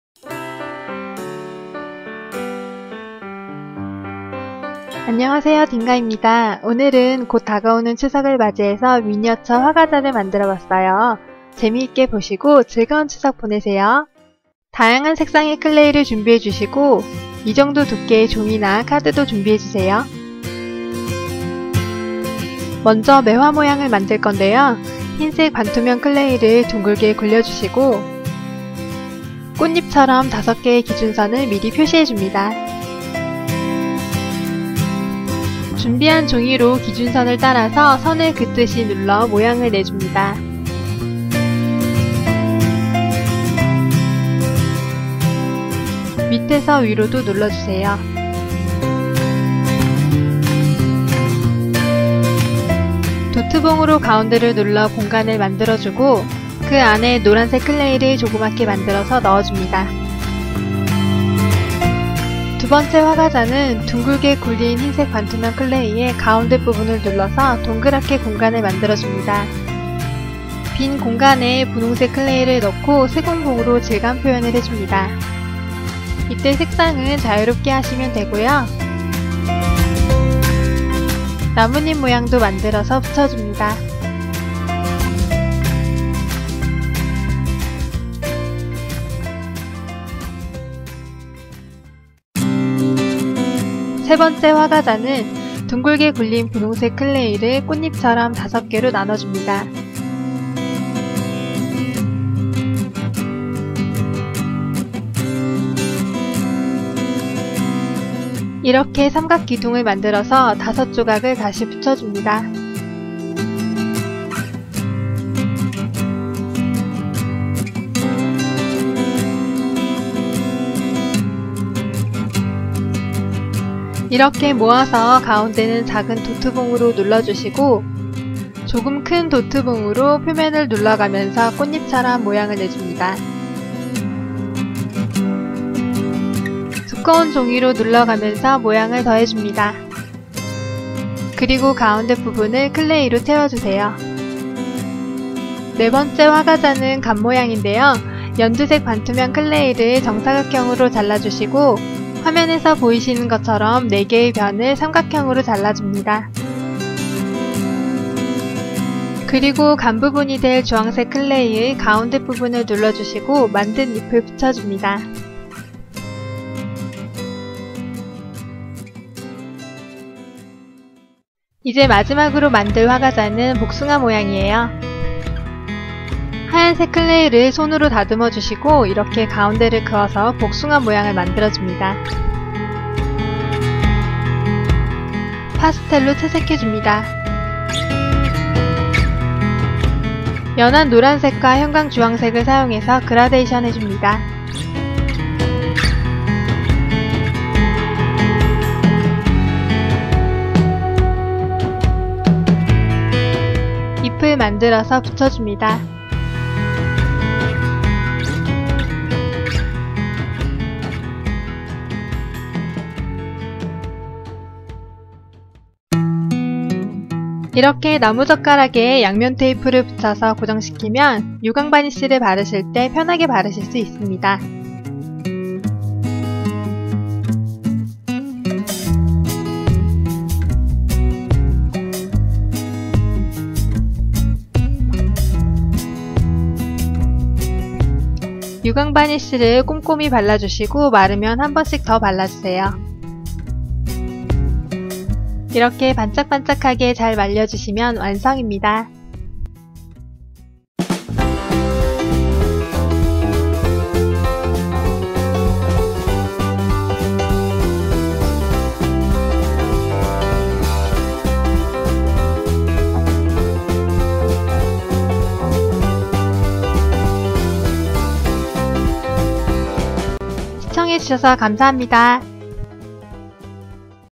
미니어쳐 화과자 [추석특집] Miniature Wagashi / 딩가의 회전목마
안녕하세요 딩가입니다.
오늘은 곧 다가오는 추석을 맞이해서 선물용으로도 좋은~
귀여운 미니어쳐 화과자를 만들어봤어요
간단하면서도 예쁘게 만들어보실수있게 5가지모양과 방법을 준비했습니다.
그럼 즐거운 명절 보내세요  감사합니다.